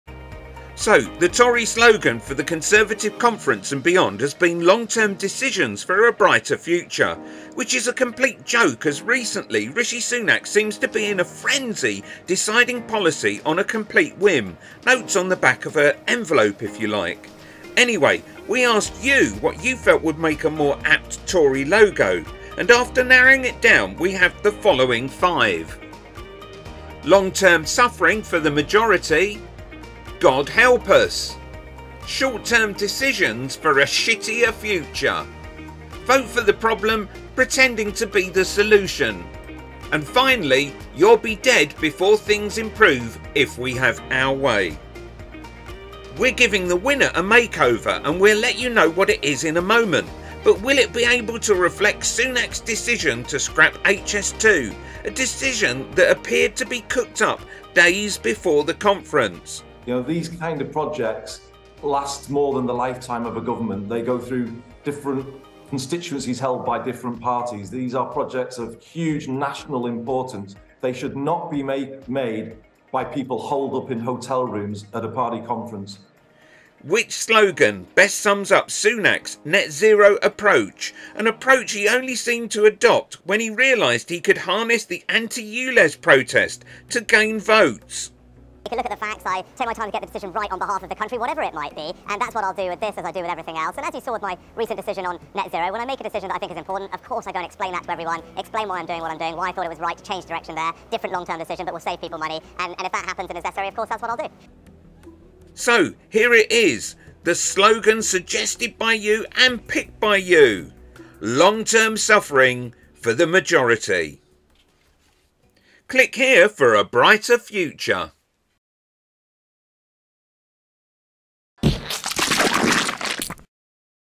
REPOST: You Voted For This To Be The New Tory Slogan...
We asked could you create a better more apt slogan for the Tories than: Long term decisions for a brighter future... Because at the moment it seems Rishi is trying out whatever fits, making the Tories look like an outfit that's totally  ramshackle. Does the slogan you suggested and voted for fit the bill? Let's see...